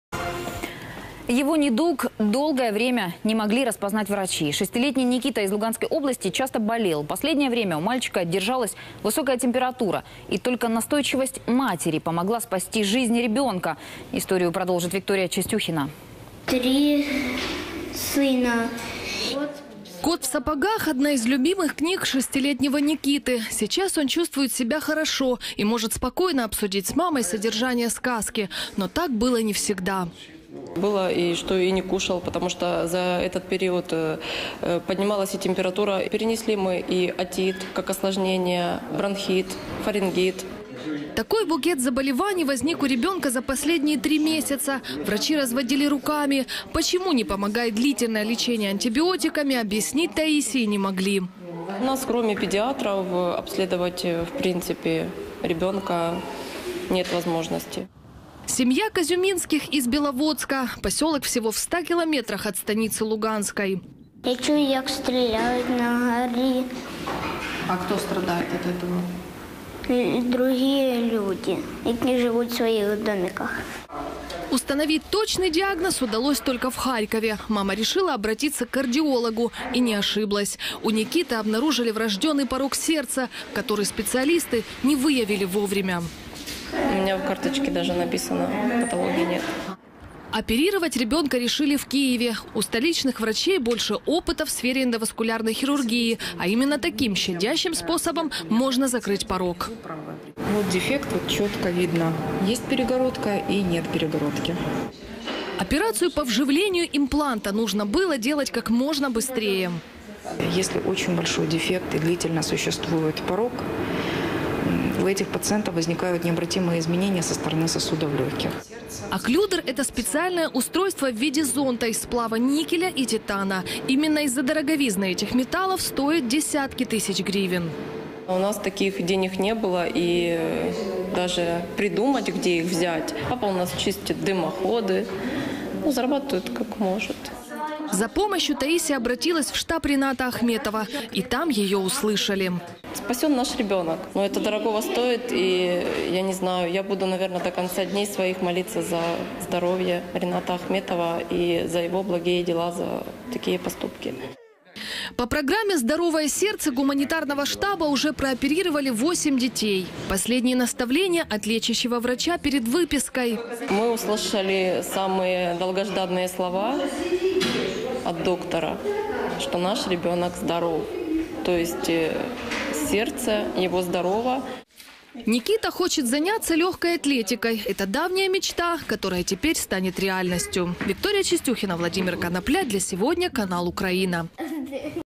Его болезни не могли распознать врачи
Его недуг долгое время не могли распознать врачи. Шестилетний Никита из Луганской области часто болел. Последнее время у мальчика держалась высокая температура. И только настойчивость матери, помогла спасти жизнь ребенка.